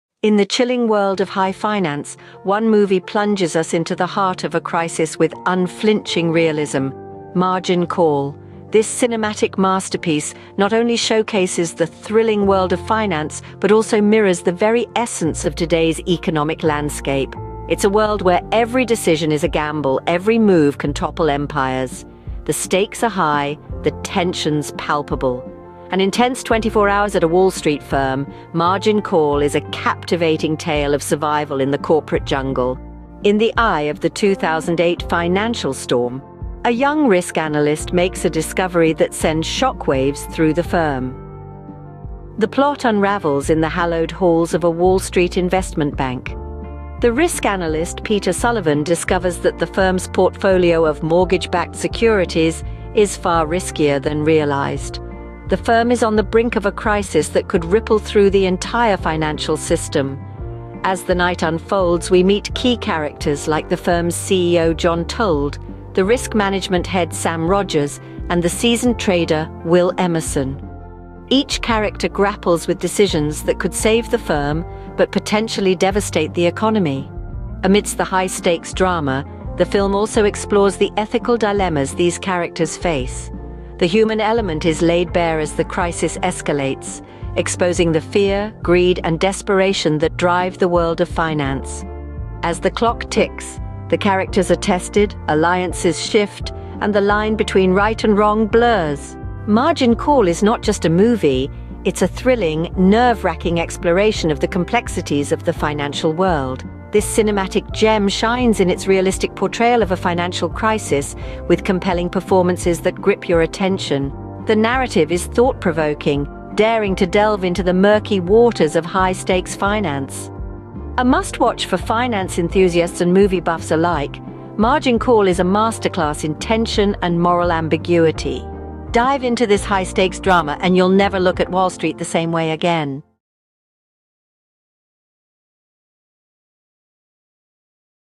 Unveiling the Financial Meltdown: 'Margin Call' Review | Explore the intense world of Wall Street
Unveiling the Financial Meltdown: 'Margin Call' Review 📈🎬 | Explore the intense world of Wall Street's darkest hour in this movie review. A riveting dive into the 2008 crisis, dissecting the ethical quandaries that unfolded. MarginCall FinancialCrisis MovieReview"

"Margin Call" offers a gripping portrayal of the 2008 financial crisis, diving deep into the moral dilemmas faced by a Wall Street investment bank. Stellar performances by an ensemble cast and a tense narrative make it a compelling exploration of corporate greed and the consequences of unchecked risk.